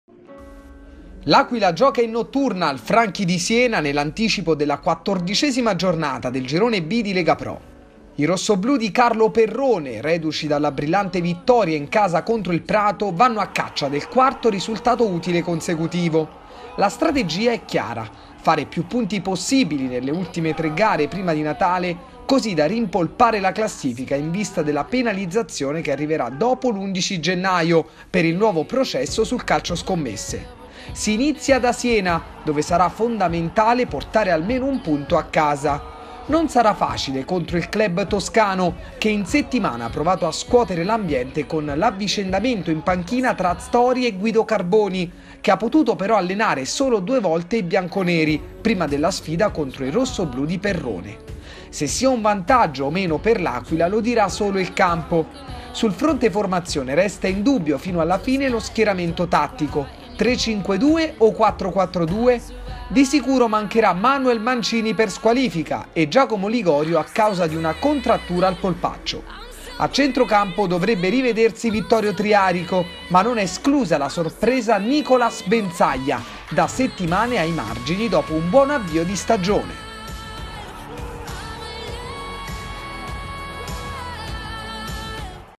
L’Aquila Calcio: prova di maturita’ contro il Siena
Domani, alle 20.30, i rossoblù saranno di scena all’Artemio Franchi di Siena contro una delle squadre più attrezzate del torneo. Obiettivo: centrare il quarto risultato utile consecutivo